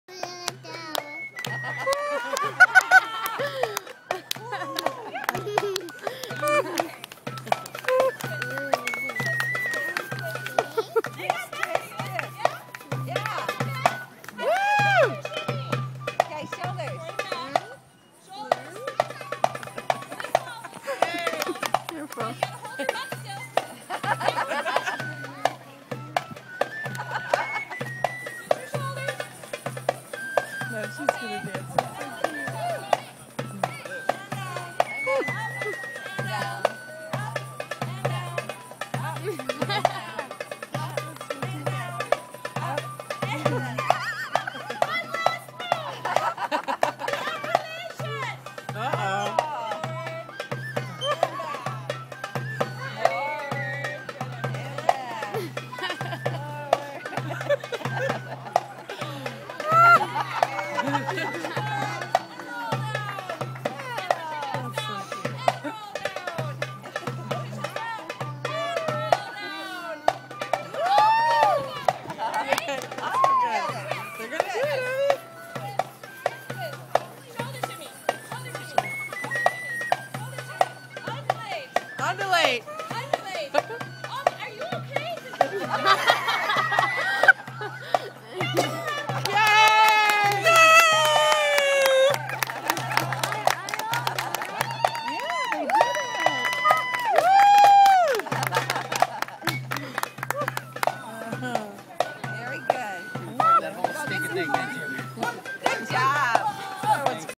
Chris dancing at the renaissance fair
Chris dancing at the renaissance fair. Enjoy!